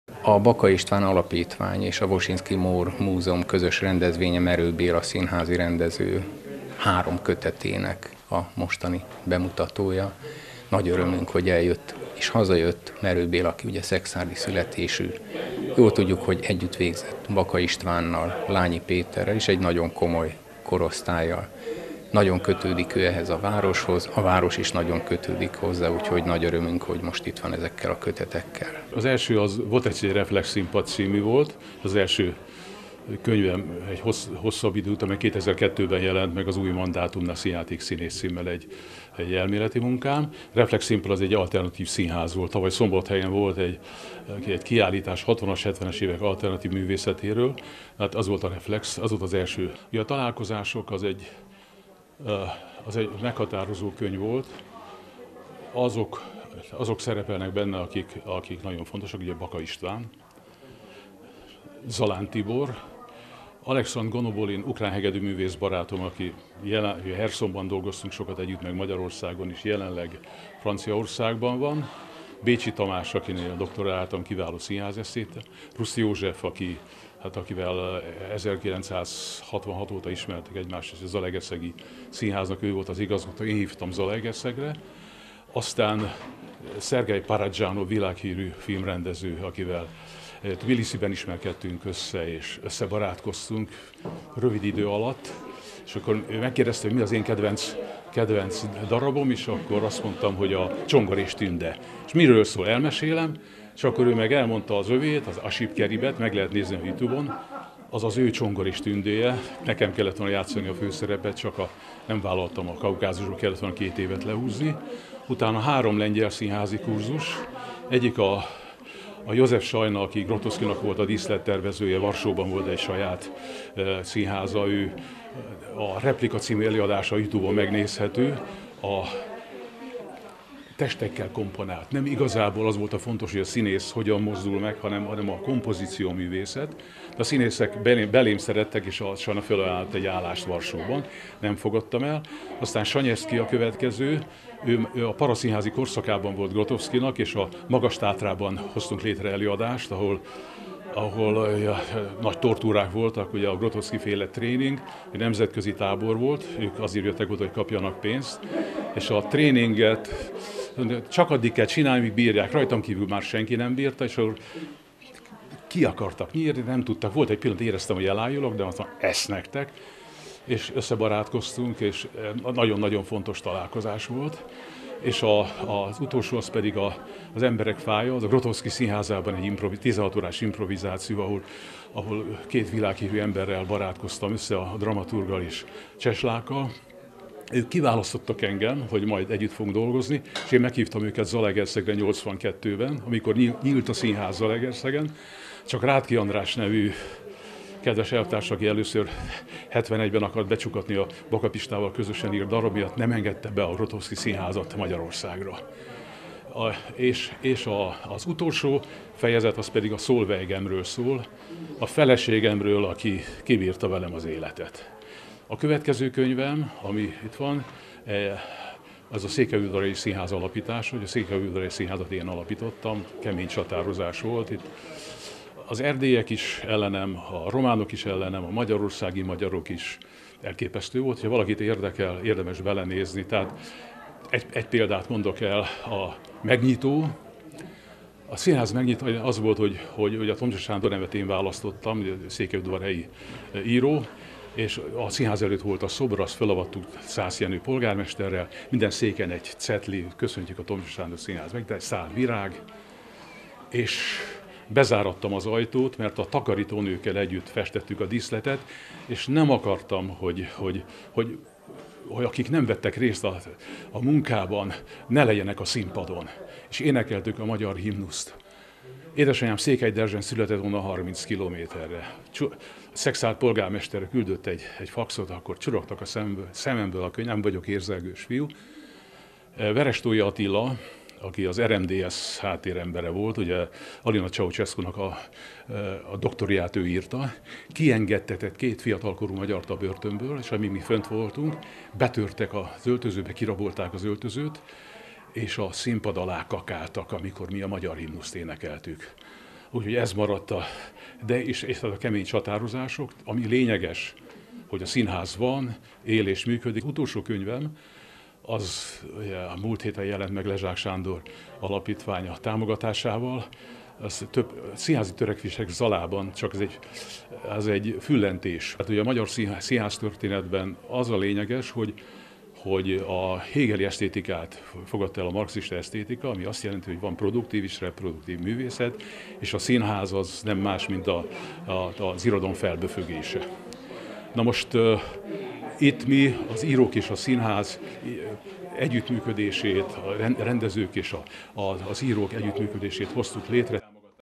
Merő Béla színházi rendező - kötetbemutatói a szekszárdi Irodalom Házában
A szekszárdi születésű színházi rendezővel beszélgettünk köteteinek bemutatója kapcsán. Merő Béla az Irodalom Házába hozta el a Találkozások, Egy színház alapítás viszontagságai és Színházi törekvések Zalában című alkotásait.